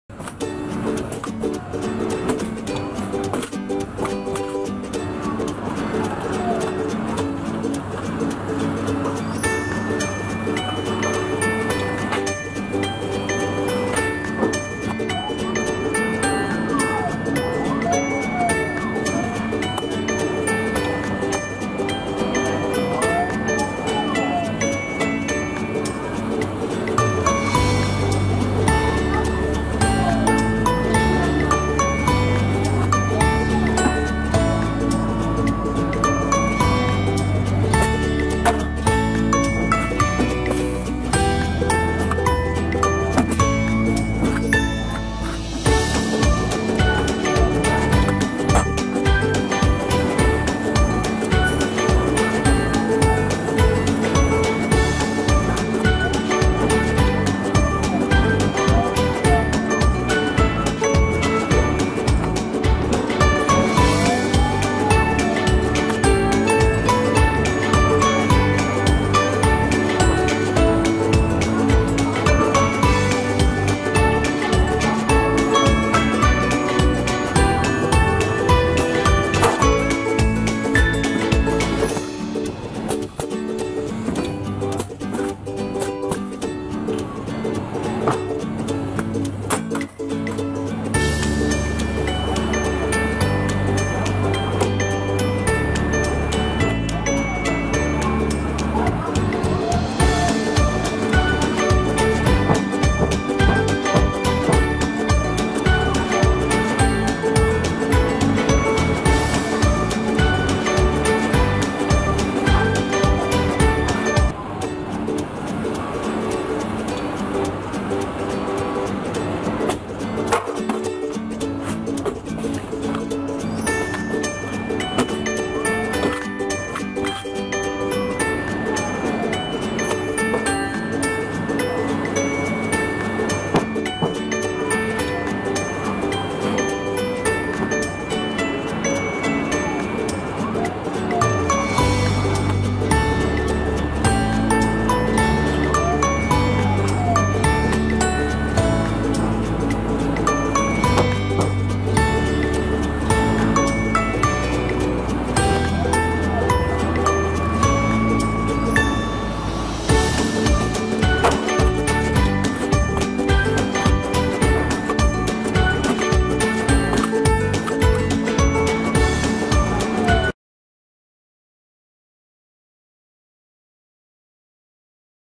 How to Weld Stainless Steel & Stainless Steel Fittings Automatically
How to Weld Stainless Steel & Stainless Steel Fittings Automatically. YSGLASSHARDWARE specializes in manufacturing all kinds of stainless steel products. Have a surprise you can't expect, web